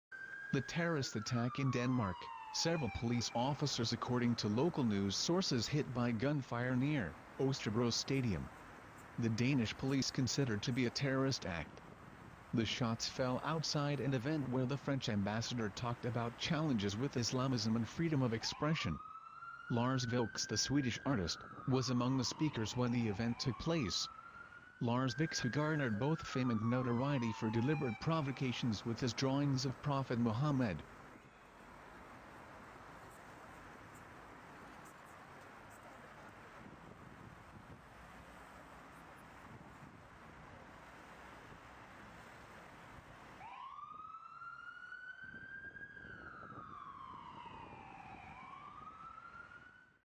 Terrorist Attack in Denmark
The terrorist attack in Denmark. There officers are confirmed shot. 2015/02/14
The shots fell outside an event where the French ambassador talked about challenges with Islamism and freedom of expression. Where Lars Vilks was guest speaker at the meeting, which took place in in Copenhagen.

Swedish artist, who garnered both fame and notoriety for deliberate provocations, with his drawings of prophet Muhammad, which resulted in at least one failed attempt by offended extremists to murder him.

The police confirms this as an act of terrorism .

There were armed police presence prior to the event.
Feel Free to use the Video!

Update:

A 40-year-old man is reported killed after a shooting at Oesterbro Saturday afternoon in Copenhagen.

Three officers are also injured. Police said that officers are not fatal wound. The relatives are according to police informed.

Police were searching for a dark VW Polo. The car is according to the police now found on Borgervænget in Copenhagen.

Police still looking for two two assailants and asking the public for help.